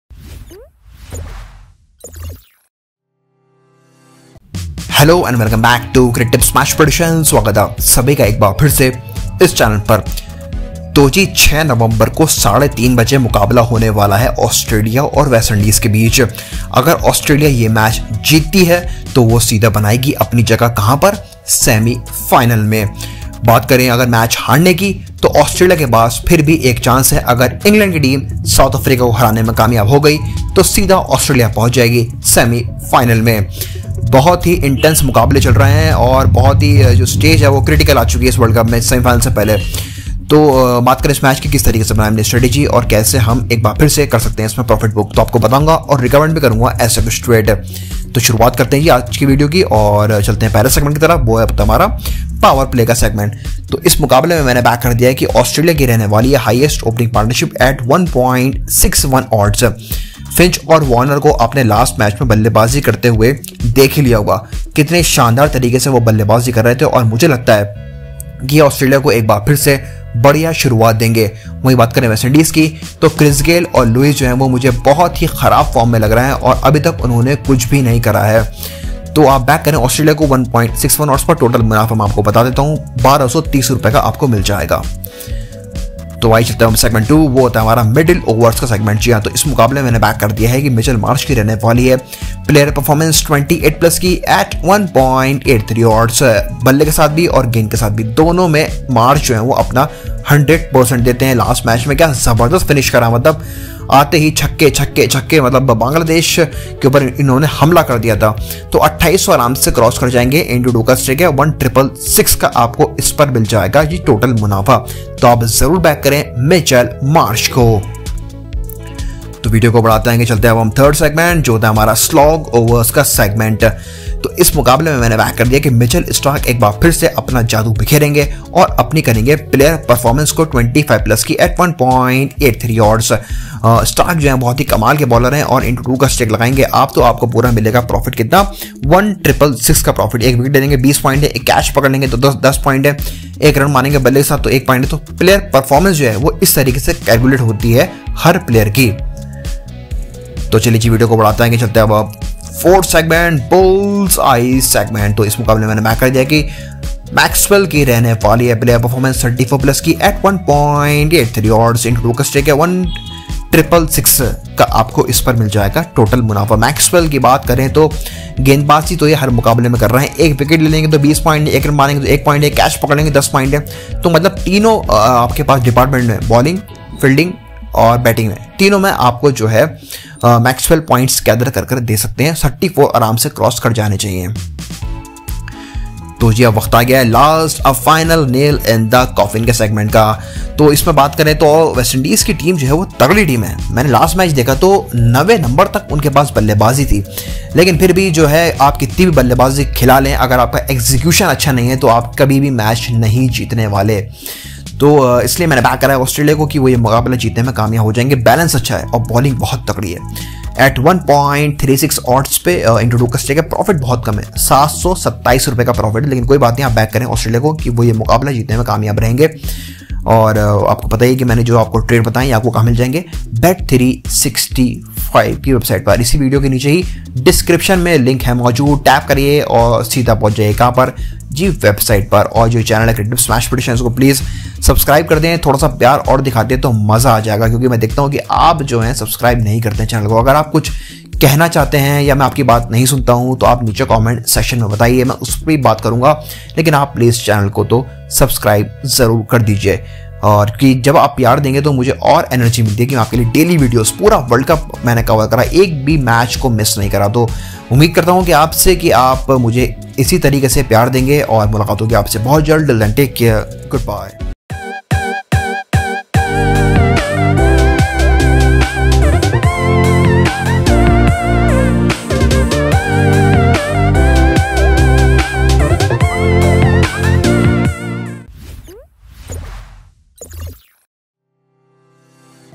AUS vs WI | World T20 2021 | Match Prediction | Match 38
The Video is all about World T20 2021 and Match Prediction on Match 38 of T20 World cup 2021 which will be played between AUS vs WI  on the 6th Nov 2021 at 03:30 PM

💰  WELCOME BONUS OFFER -  15% Welcome Bonus up to ₹5,000 💰
Netbanking, Mastercard and VISA
Safe deposits and withdrawals

00:00 - Intro
00:53 - Powerplay segment
01:26 - Middle overs Segment
02:00 - Slog Overs Segment
02:30 - Bulls Eye Segment
03:08 - A Final Nail In The Coffin